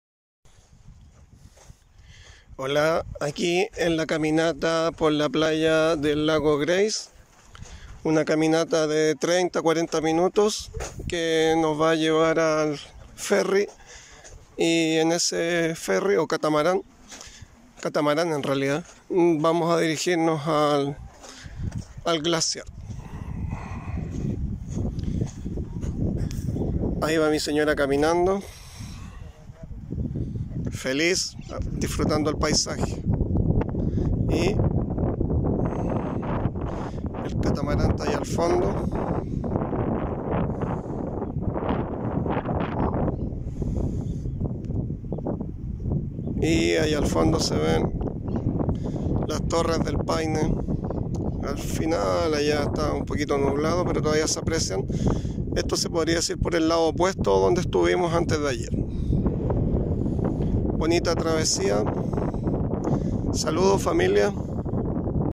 Lago Grey, caminata por la playa.
Luego de llegar al hotel Grey, en parque nacional torres del Paine, se camina 40 minutos para tomar el catamaran que te llevará por el lago Grey y sus diferentes y hermosos glaciares.